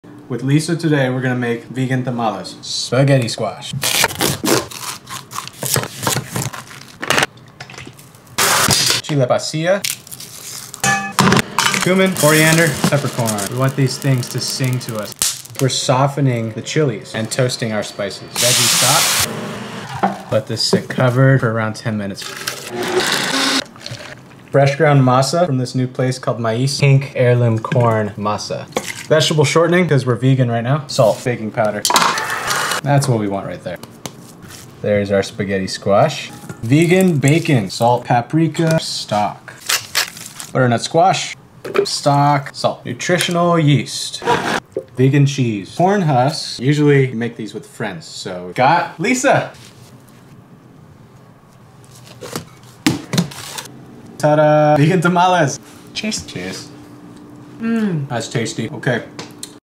Lisa Nguyen Stuffs My Tamale 🫔
Making Tamales with @LisaNguyen

Dough)
-3C Masa Harina
-1C shortening
-2C veggie chili stock
-1 tbsp salt.
-1tbsp baking powder
(Mix all ingredients and keep covered)

-1 spaghetti squash
-oil to coat
-salt pepper to taste
(Roast squash til for tender)

(For ‘cheese’)
-butternut squash about a pint roasted
-veggie stock til smooth
-salt to taste
(Blend all til smooth)